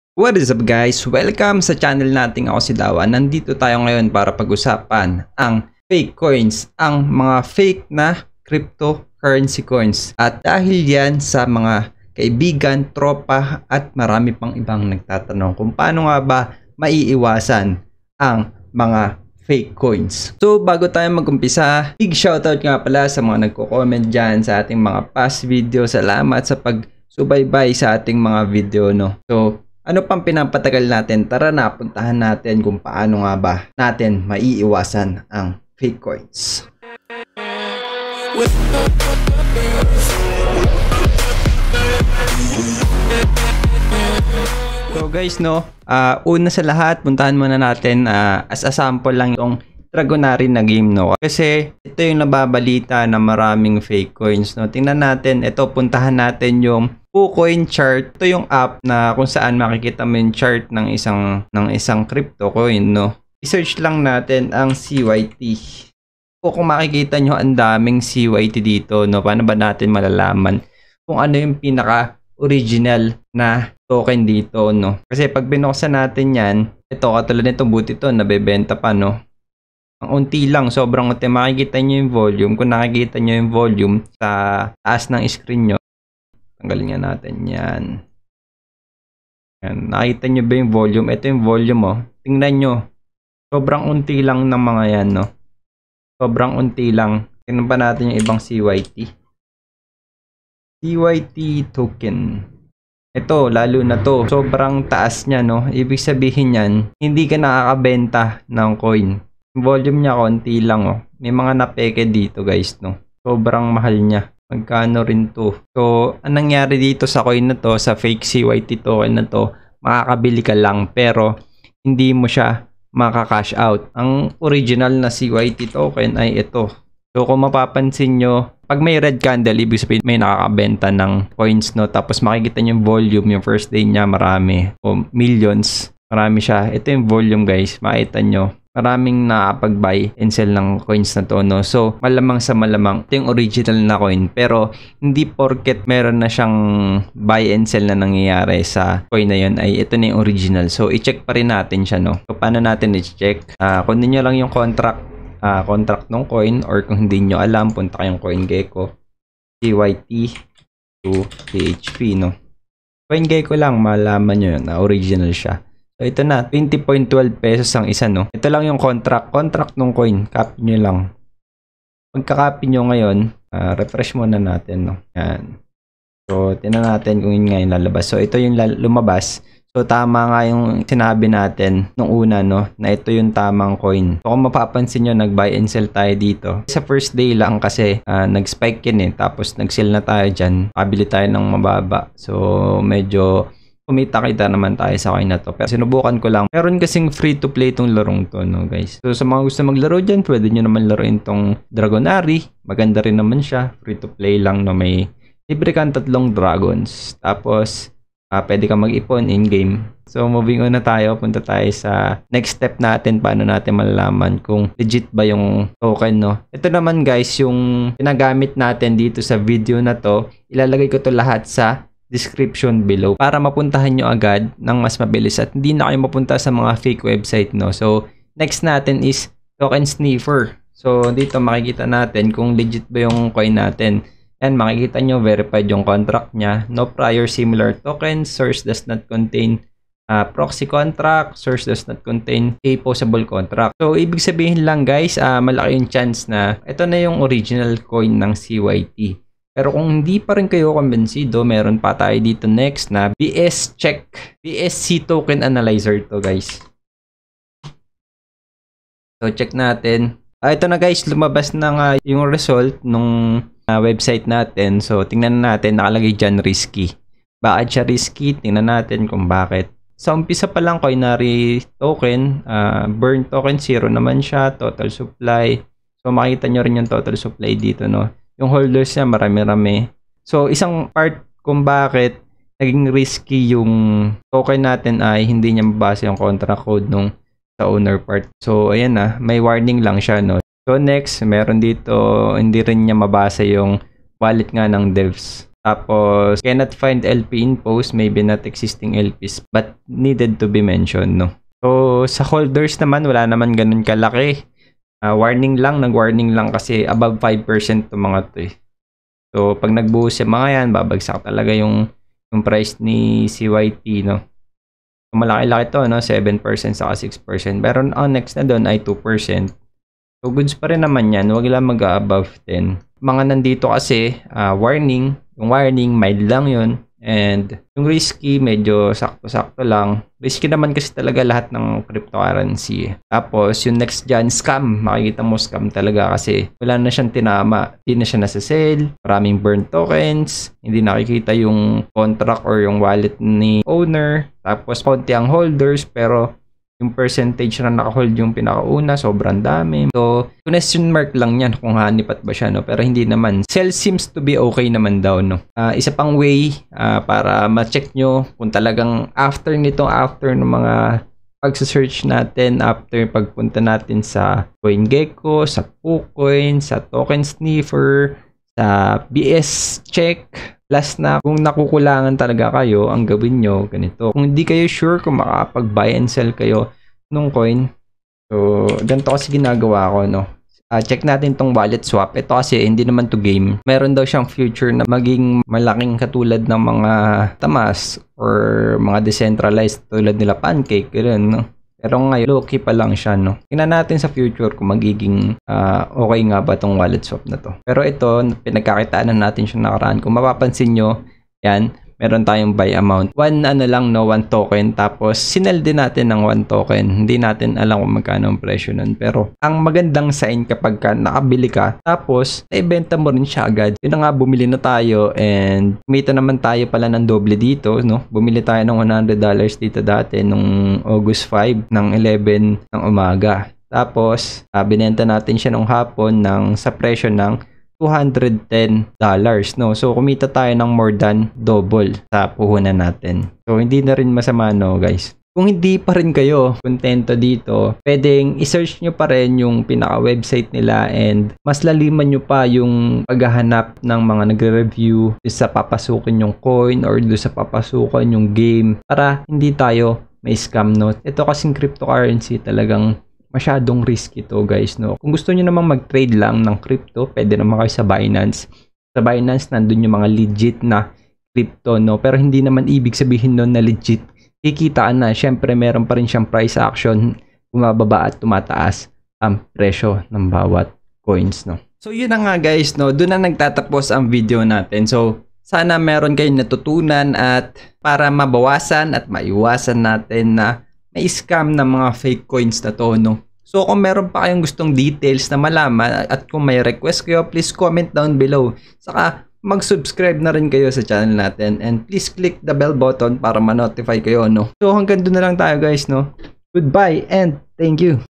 HOW TO SPOT AND AVOID FAKE CRYPTO COINS (PAANO NGA BA MAIIWASAN?)
Natatakot ba kayong maScam ng Fake Coins?
Ang video na ito ay para sa inyo!